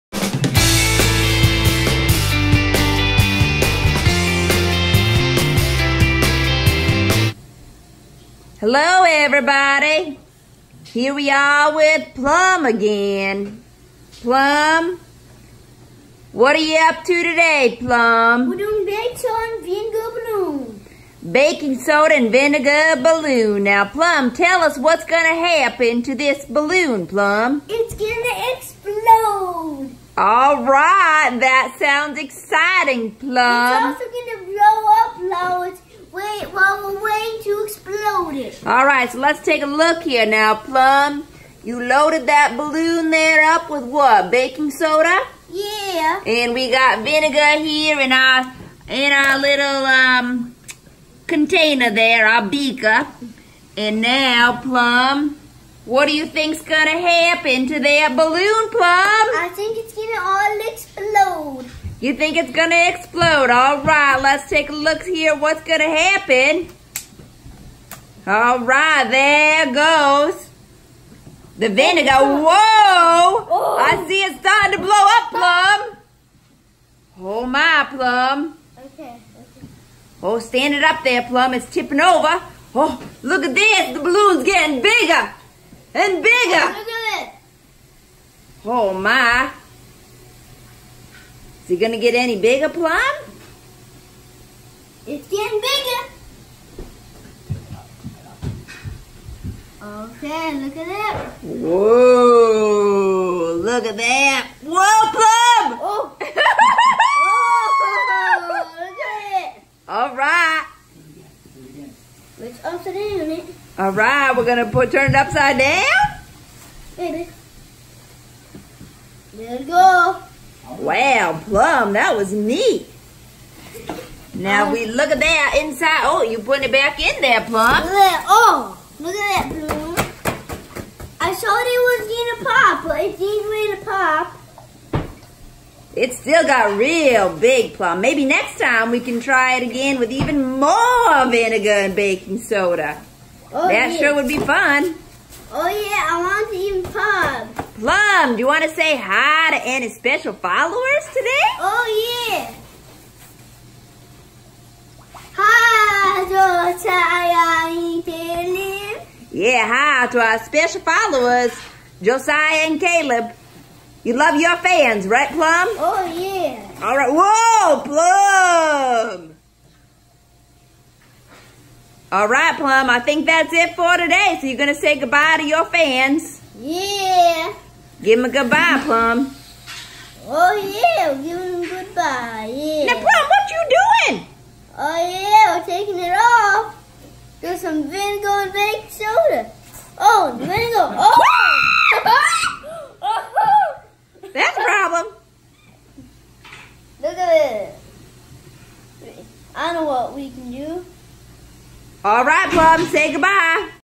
Blowing Up A Balloon With Baking Soda And Vinegar!
Plum blows up a balloon with baking soda and vinegar!